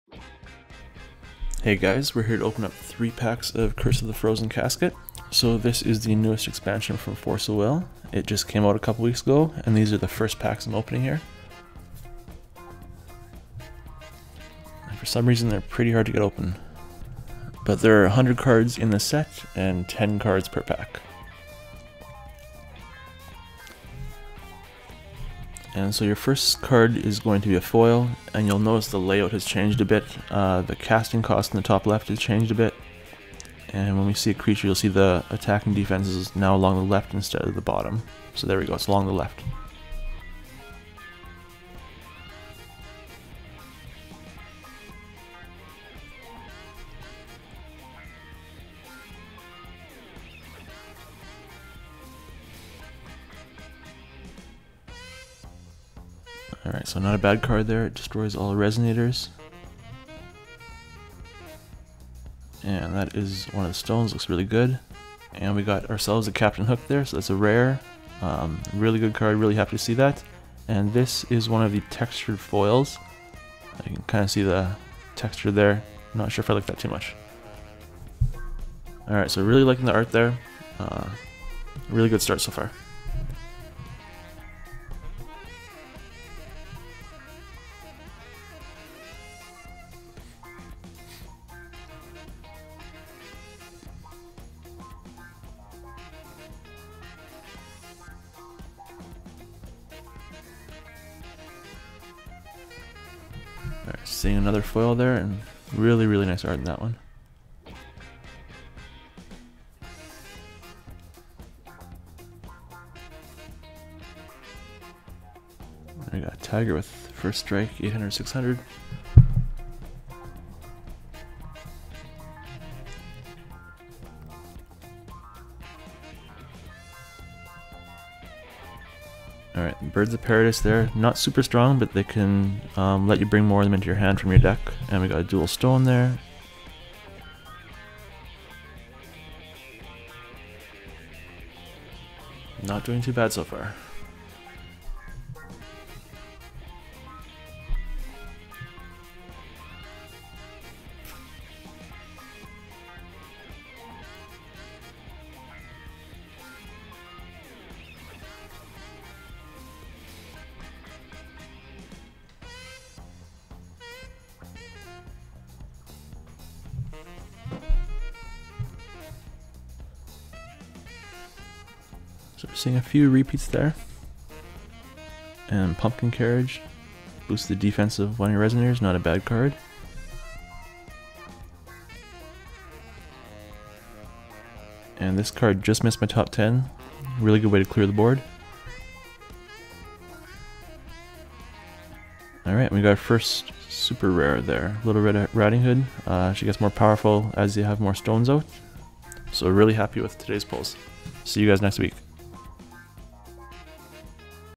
Force of Will - Curse of the Frozen Casket Booster Pack Stat 1 - 3
Join me as I open up a few packs of Force of Will Curse of the Frozen Casket. I'm going to track the cards that I pull and see how many packs I need to open before I get 95% of the cards in the set.